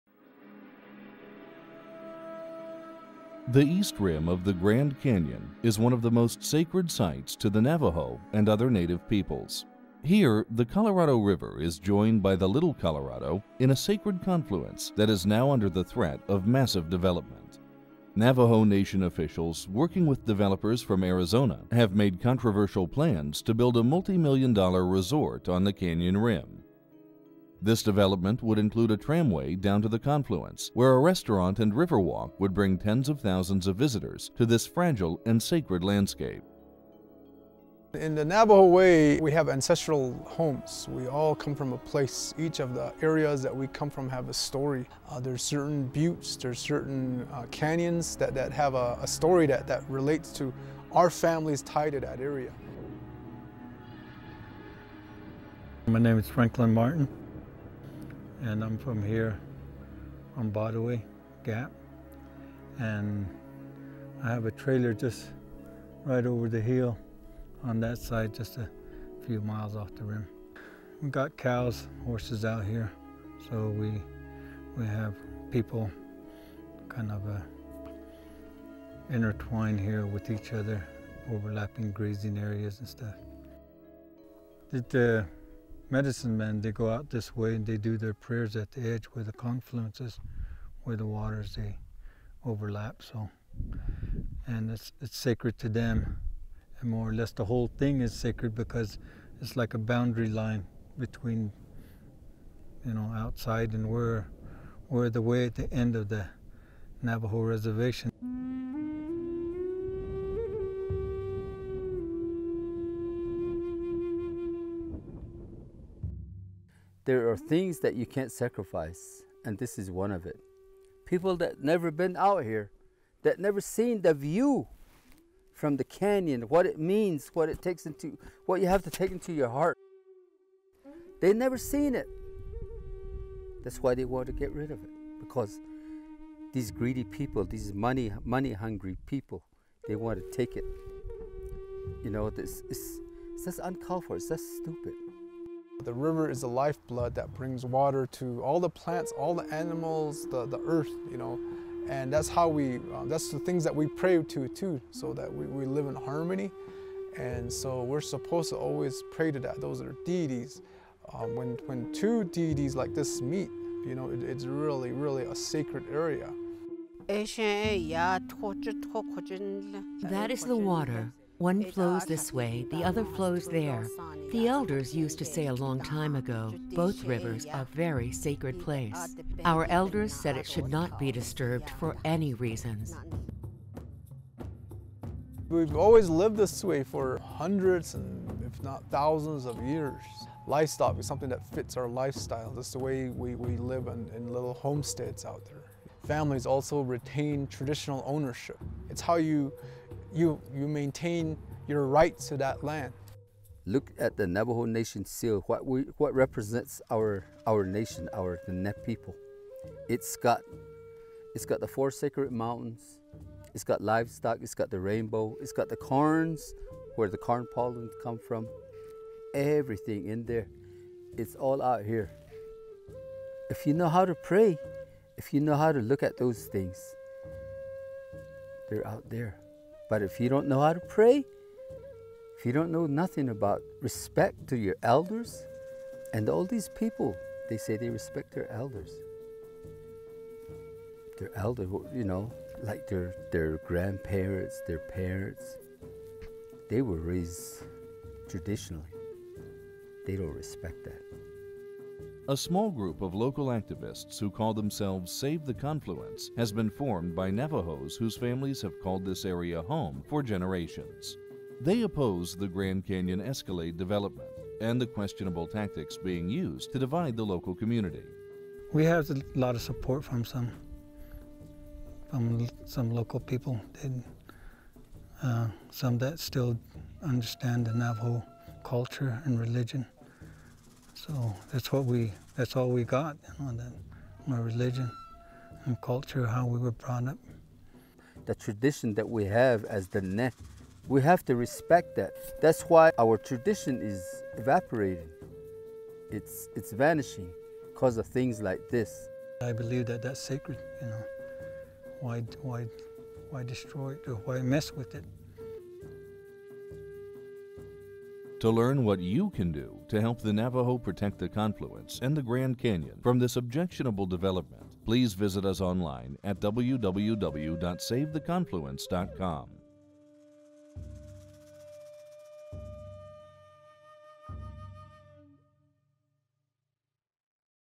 Save the Confluence
The Navajo group "Save the Confluence" are opposed to the development of the Grand Canyon Escalade project at the confluence of the Colorado and Little Colorado Rivers on the Navajo Reservation in Arizona. This video explains why this area is sacred and should be preserved in its natural state.